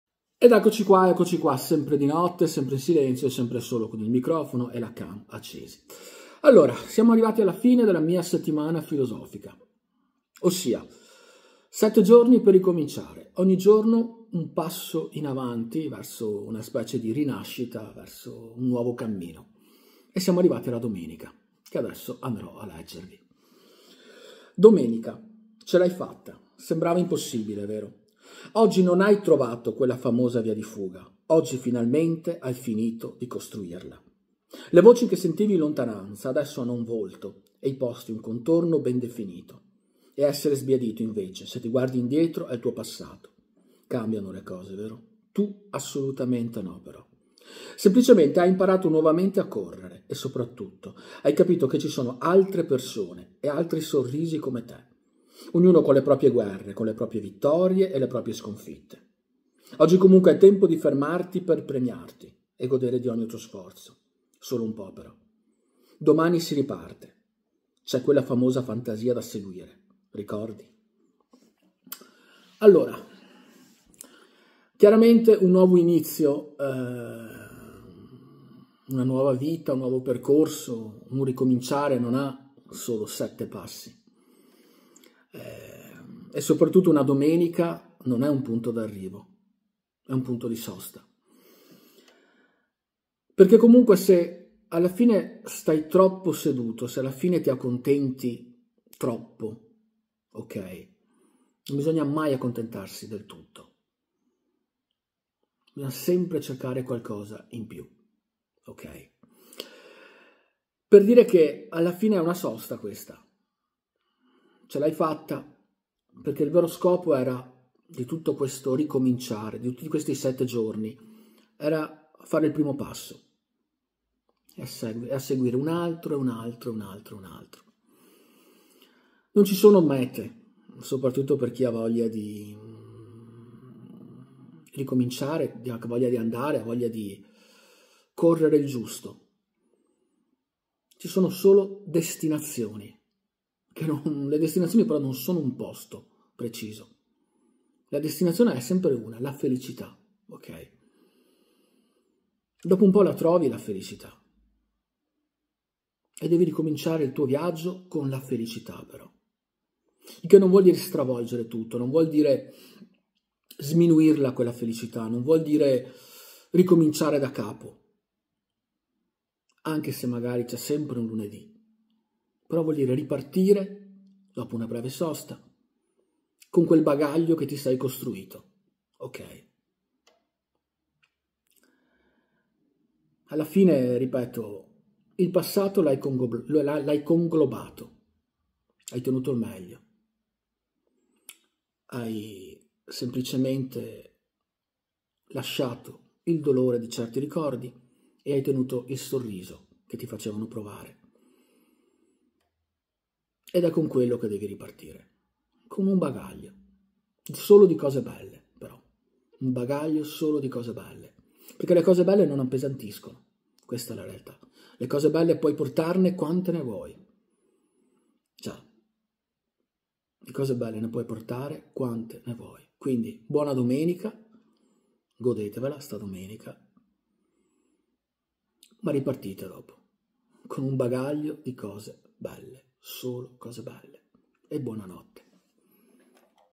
Monologo motivazionale : Buona domenica! 4tu
E siamo arrivati alla fine della mia settimana filosofica!
Buona domenica!

Testo (Parole Fortunato Cacco)
Ce l’hai fatta! sembrava impossibile vero…?

Oggi non hai trovato quella famosa via di fuga….oggi finalmente hai finito di costruirla!

Le voci che sentivi in lontananza adesso hanno un volto e i posti un contorno ben definito e a essere sbiadito invece se ti guardi indietro è il tuo passato…

Cambiano le cose vero?...tu assolutamente no però!…semplicemente hai imparato nuovamente a correre e soprattuto hai capito che ci sono altre persone e altri sorrisi come te…ognuno con le proprie guerre, con le proprie vittorie e le proprie sconfitte…

Oggi comunque è tempo di fermarti per premiarti e godere di ogni tuo sforzo.

Solo un po’ però.

Domani si riparte…c’è quella famosa fantasia da seguire….ricordi?